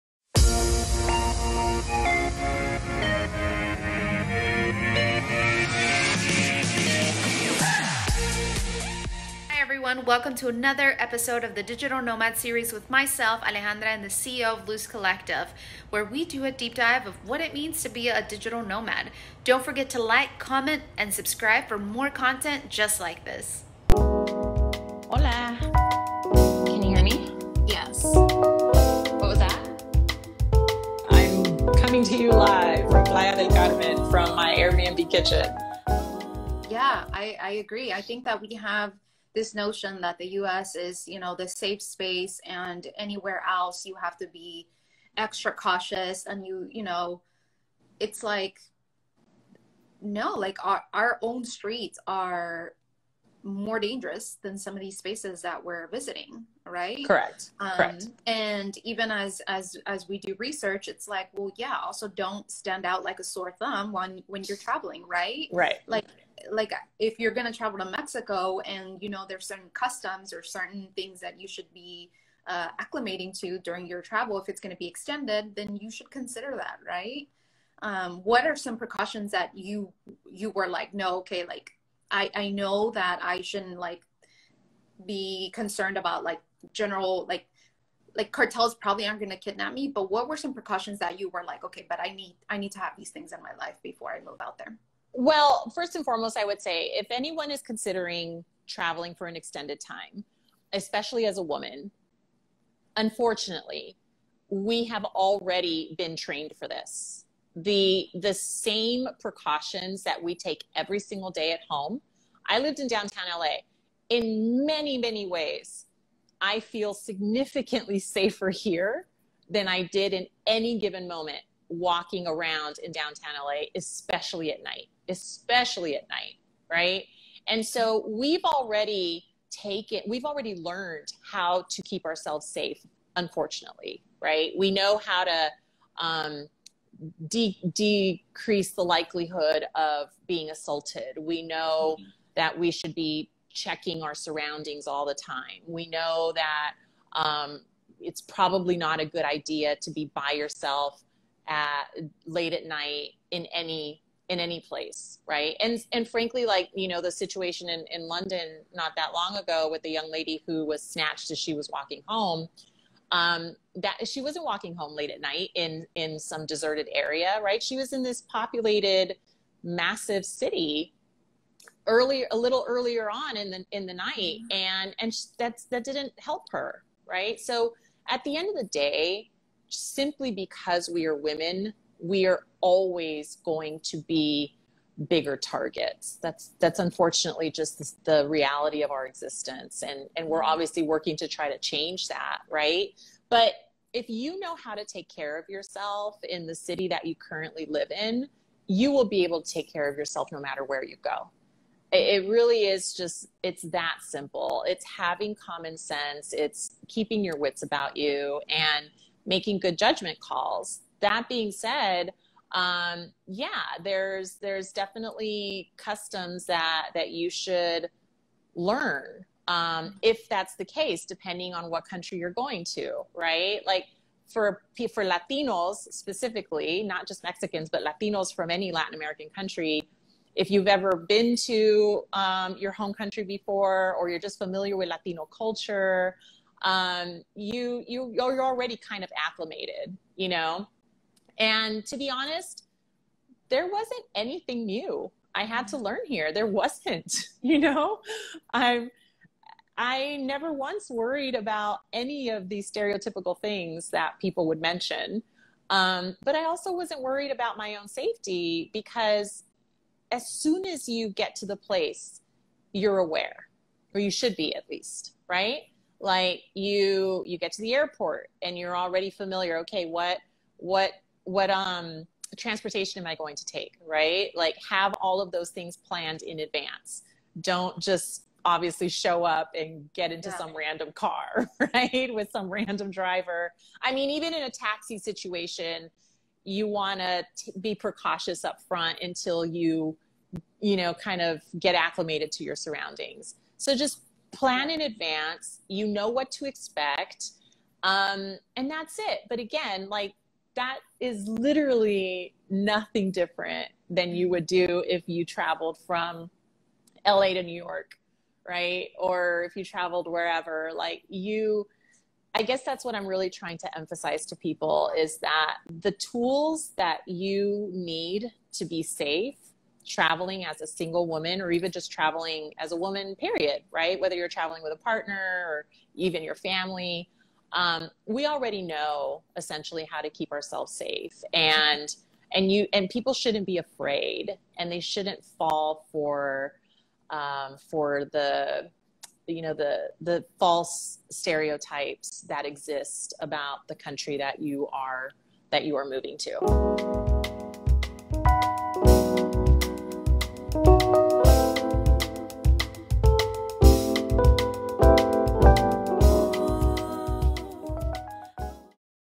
Staying Safe While Traveling as a Digital Nomad with Lucy Flores - Playa del Carmen, MX
So how "dangerous" is Mexico, really? As women, we're always having to take extra safety precautions. Layer on a pandemic to make it even more complicated. We talk about that and more with the CEO of Luz Media, Lucy Flores.
Alpha Latina: Digital Nomad Series 🏝
The inside scoop on the life of a traveling alphalatina who leads a digital media company.